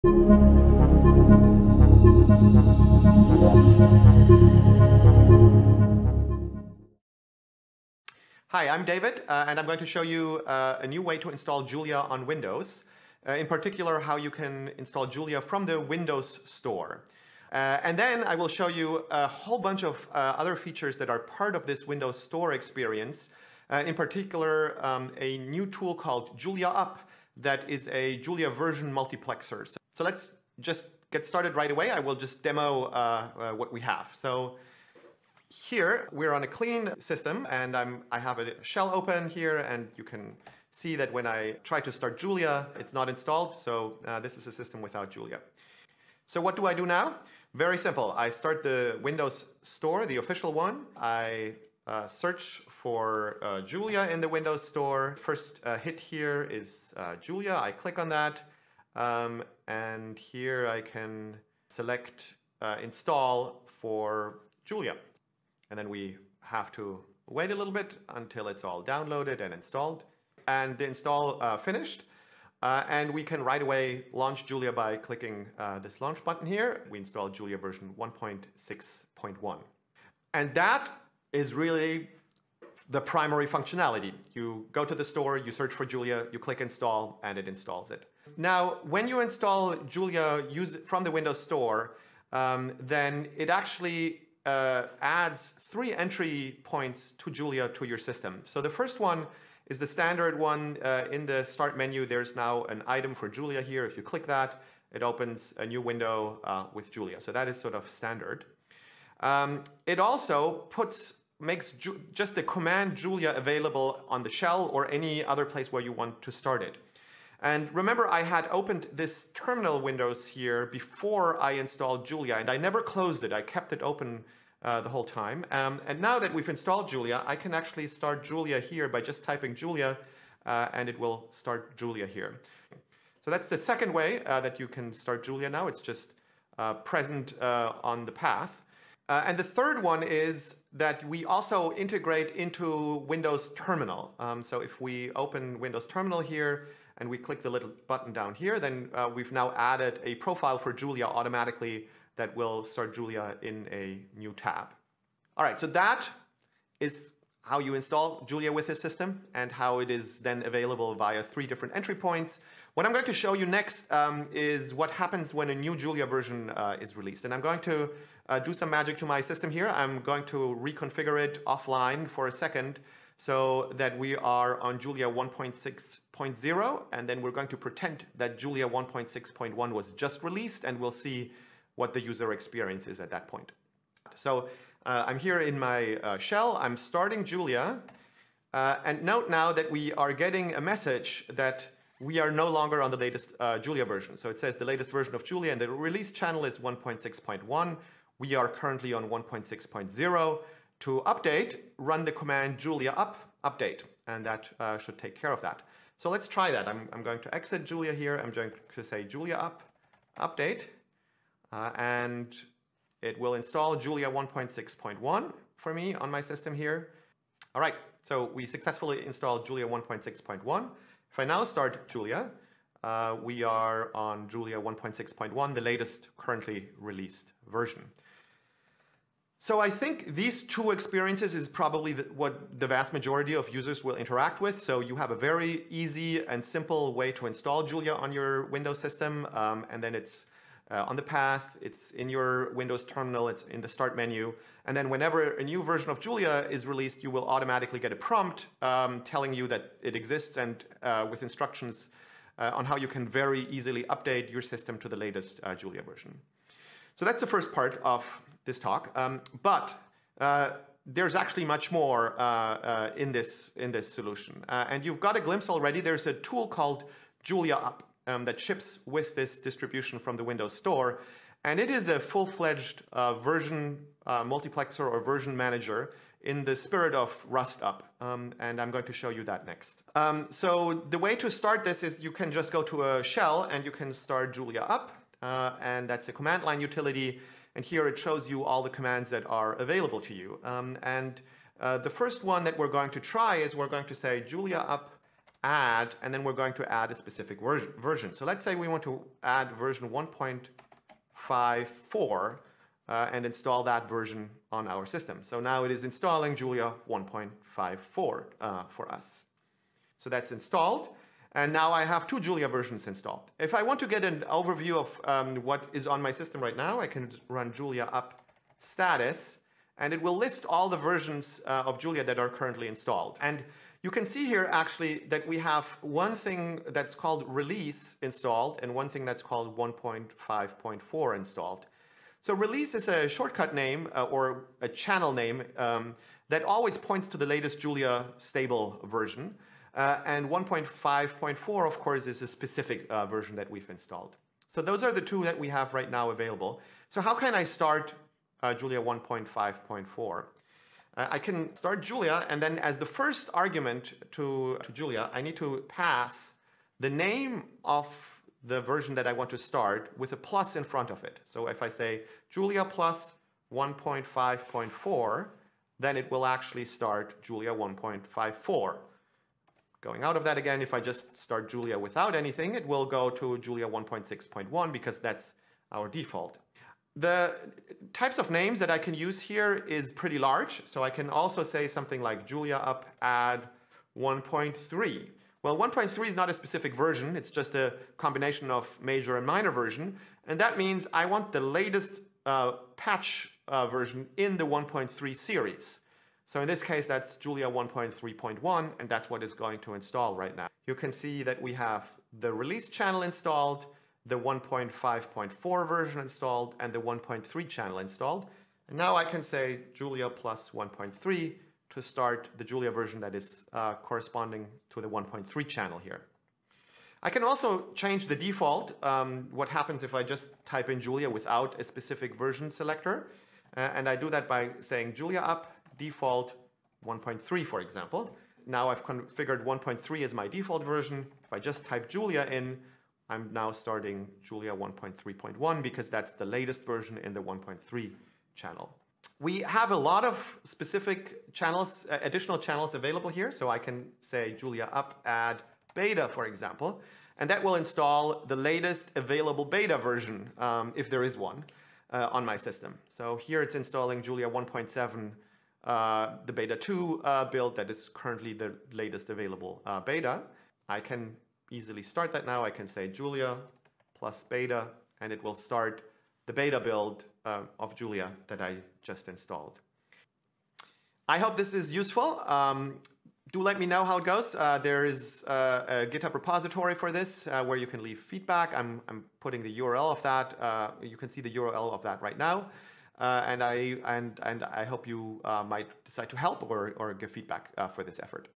Julia in the Windows Store | David Anthoff | JuliaCon2021
This talk was given as part of JuliaCon2021.

Abstract:
I will describe an effort to distribute Julia via the Windows Store. This effort includes a full Julia version manager that provides the ability to install multiple Julia versions at the same time, switch between them etc. The talk showcases an experimental working version of the installer from a user perspective, and also gives a brief deep dive around the technologies used.

00:00 Welcome!
00:10 Help us add time stamps for this video! See the description for details.